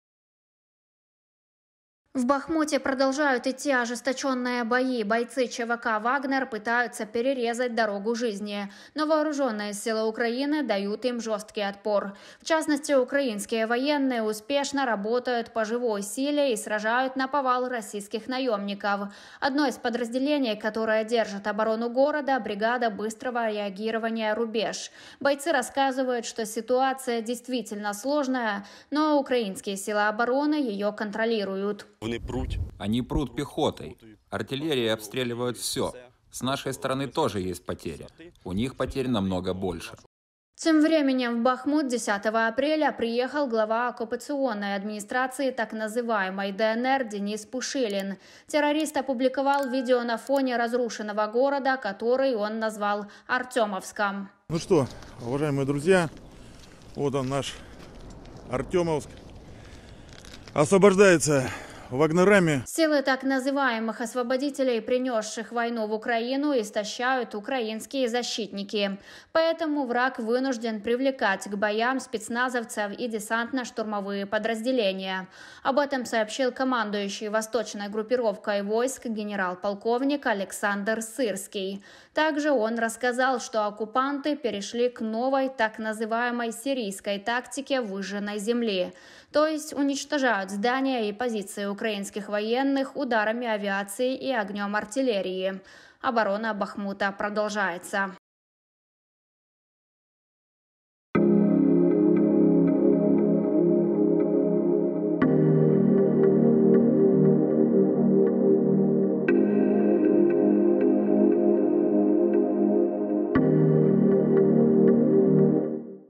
Горячие новости из Бахмута. ВСУ мощно крошат путинских орков
RU - ВСУ продолжают срывать все планы россиян и отрицательно пополнять вражескую армию в Бахмуте. Орки бесятся из-за бессилия и пробуют применять новые тактики. Детали смотрите в сюжете.

UA - ЗСУ продовжують зривати всі плани росіян та негативно поповнювати ворожу армію у Бахмуті. Орки бісться через безсилля і намагаються застосовувати нові тактики. Деталі дивіться у сюжеті.